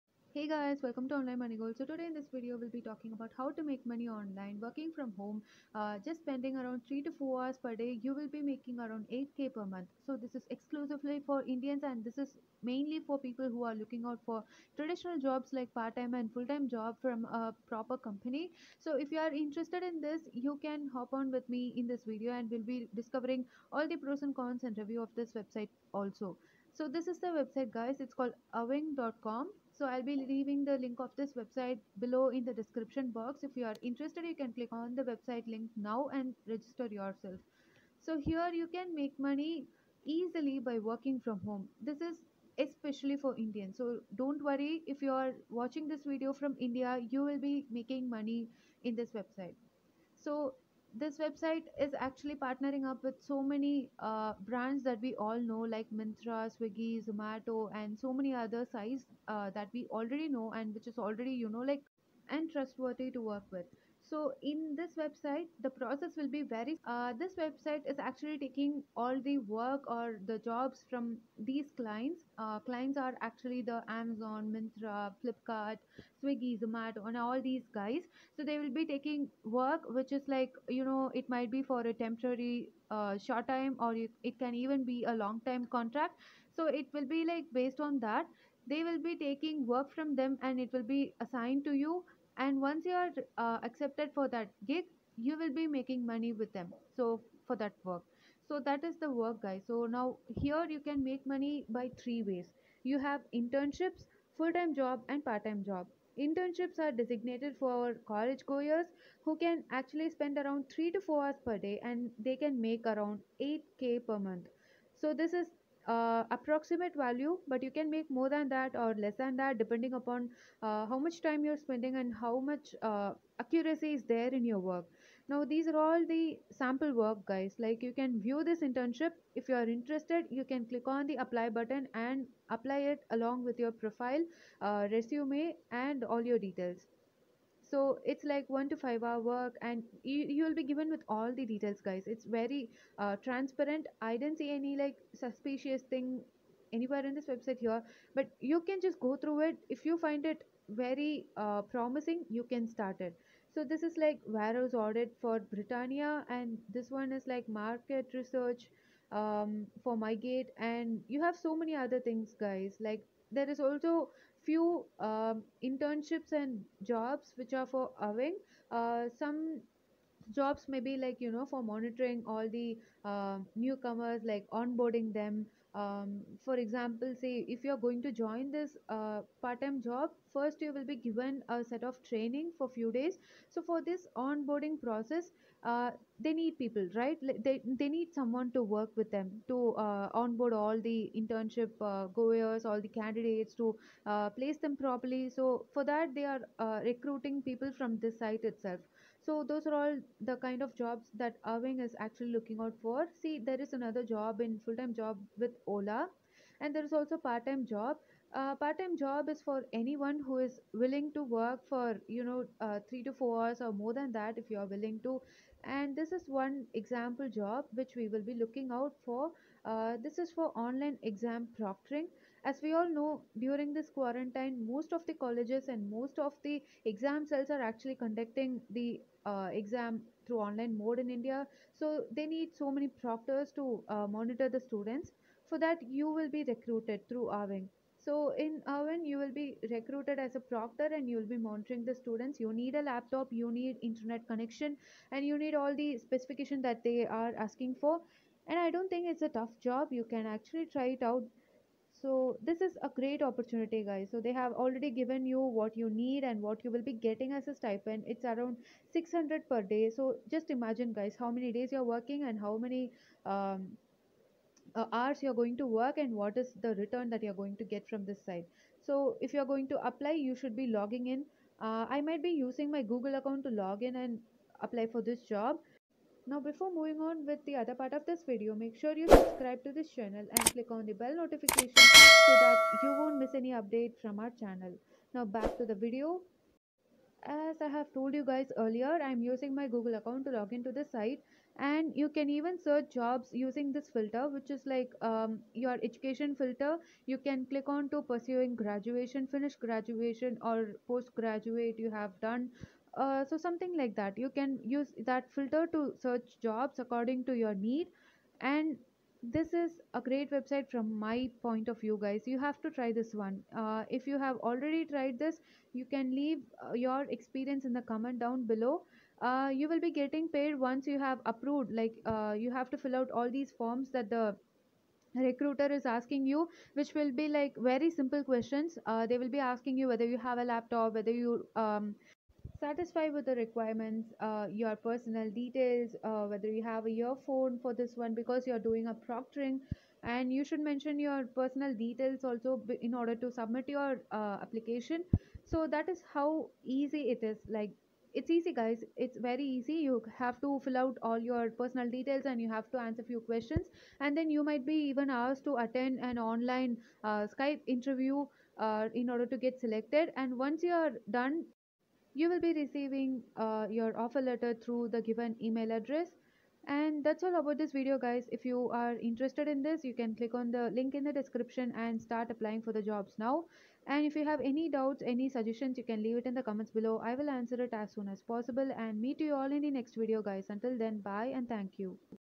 Awign work from home jobs 2021 | part time and full time jobs online | online money goals
Hey guys! in this video I will be showing you all a website called awign where you can find internships for college students,part-time and full time jobs for freshers/graduates!.This is a great opportunity to make money online with just 3-5 hrs work.Earn up to 8k per month in your internship! awign partnered with many reputed companies and provides small gigs to workers like us to make money remotely!If  you have any doubts or suggestions please leave it down below in the comments!

like the video if you want more content like this!.
I'm here to help college goers like me to earn money online to make a better living!..